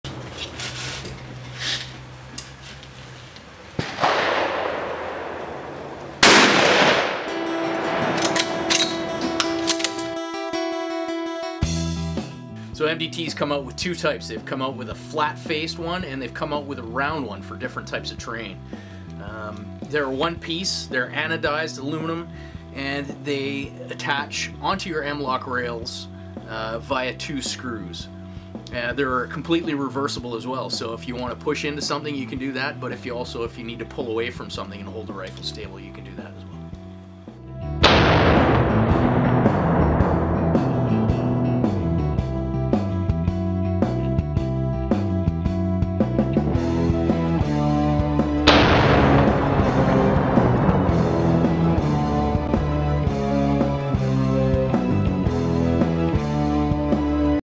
MDT M-Lok Barricade Stop
Mil-spec Type III hard-black anodized aluminum barricade stops that attach to M-LOK compatible hand guards and forends. Available in flat-face or rounded face to suit a variety of applications.